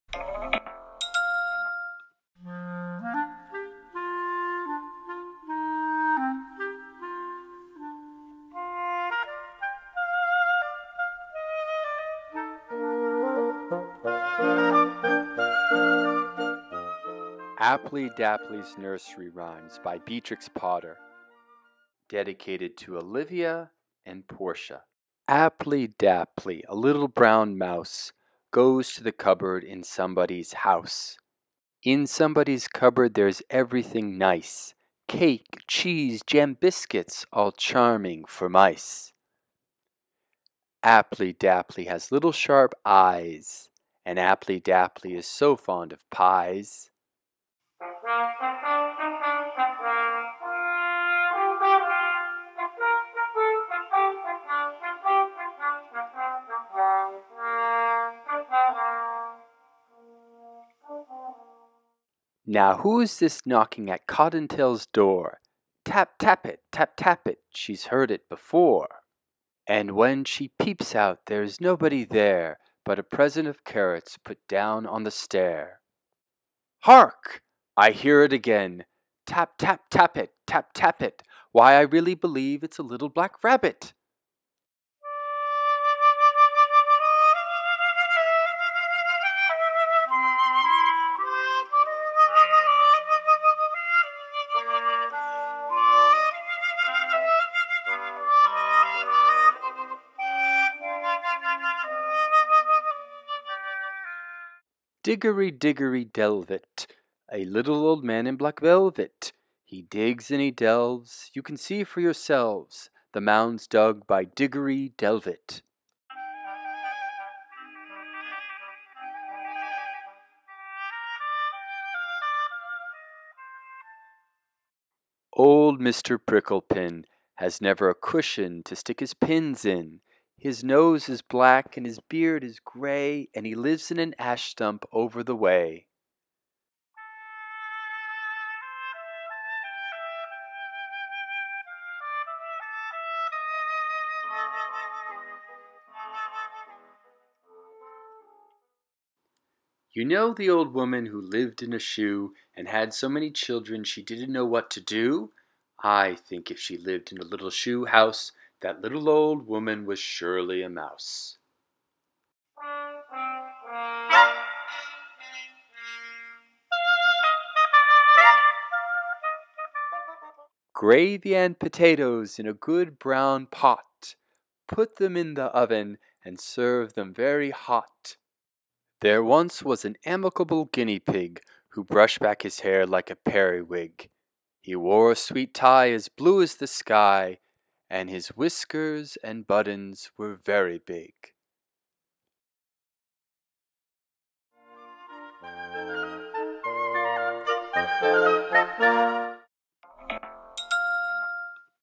GOHKIDS ~ Appley Dapply's Nursery Rhymes by Beatrix Potter ~ READ ALOUD ~ GOHACADEMY.COM
Appley Dapply's Nursery Rhymes by Beatrix Potter.
Story and illustrations by Beatrix Potter.